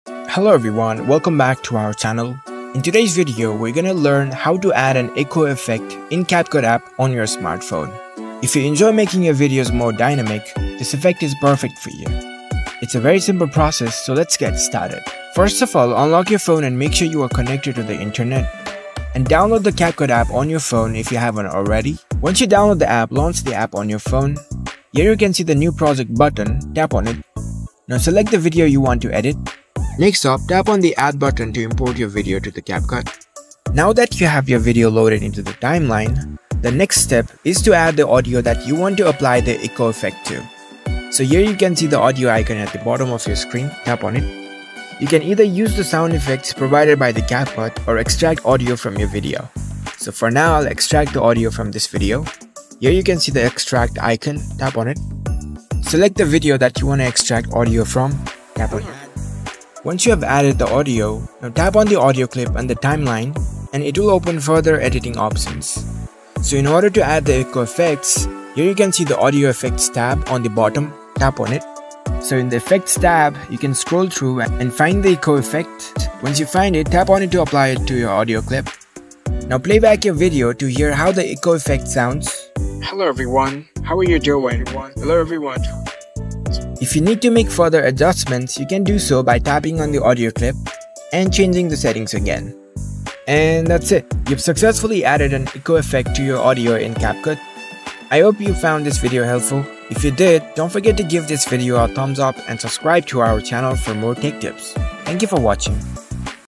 How to Add Echo Effect in CapCut 2024?
Want to make your audio stand out with an echo effect in CapCut? In this video, we’ll show you how to add an echo to your audio tracks, giving your videos a unique and professional sound

Watch as we guide you through each step to add an echo effect to your audio in CapCut.